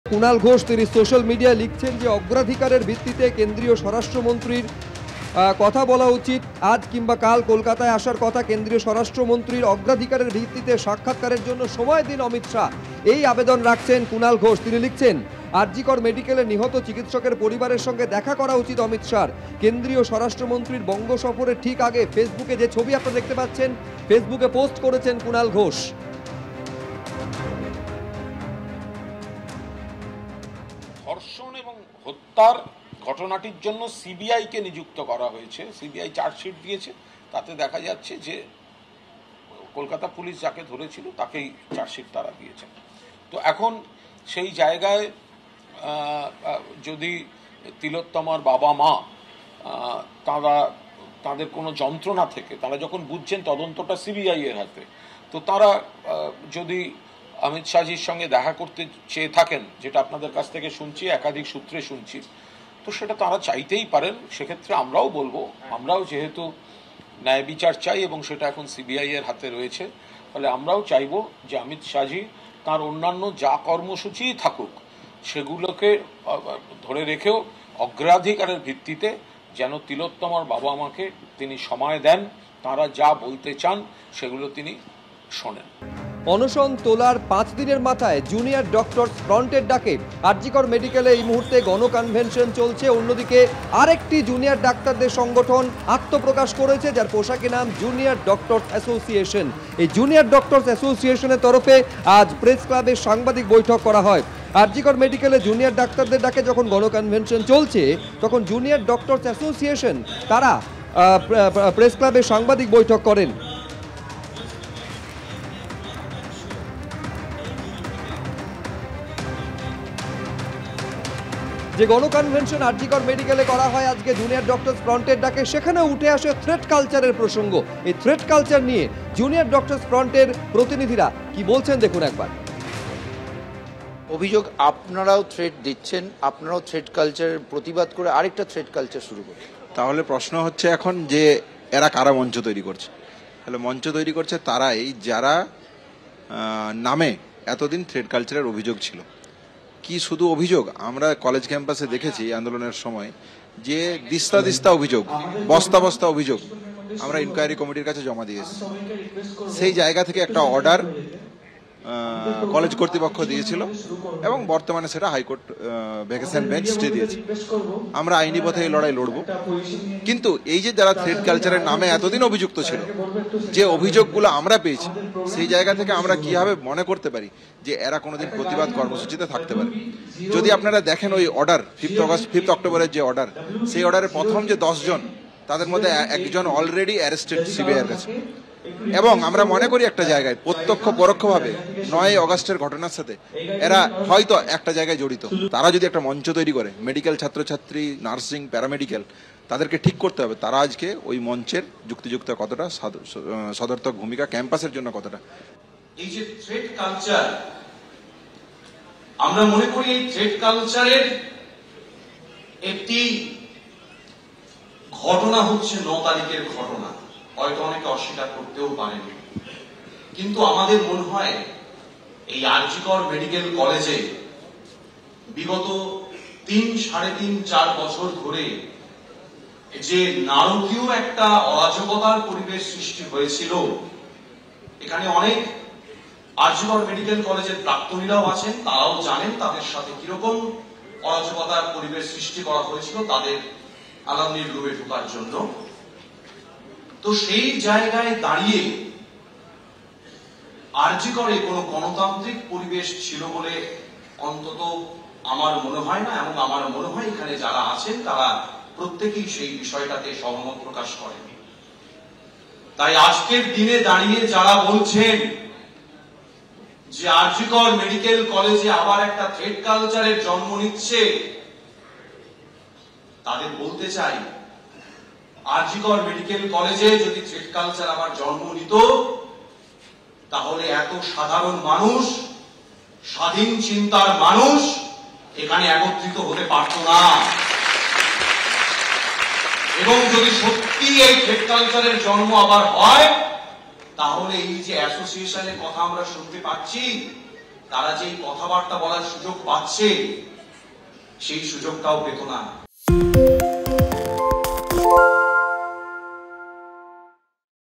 Kunal Ghosh: অগ্রাধিকারের ভিত্তিতে সাক্ষাৎকারের জন্য সময় দিন অমিত শাহ: কুণাল | ABP Ananda LIVE
ABP Ananda LIVE: 'আর জি কর মেডিক্যালের নিহত চিকিৎসকের পরিবারের সঙ্গে দেখা করা উচিত অমিত শাহের' । কেন্দ্রীয় স্বরাষ্ট্রমন্ত্রীর বঙ্গ-সফরের ঠিক আগে ফেসবুক পোস্টে দাবি কুণাল ঘোষের । আজ কিংবা কাল কলকাতায় আসার কথা কেন্দ্রীয় স্বরাষ্ট্রমন্ত্রীর । অগ্রাধিকারের ভিত্তিতে সাক্ষাৎকারের জন্য সময় দিন অমিত শাহ: কুণাল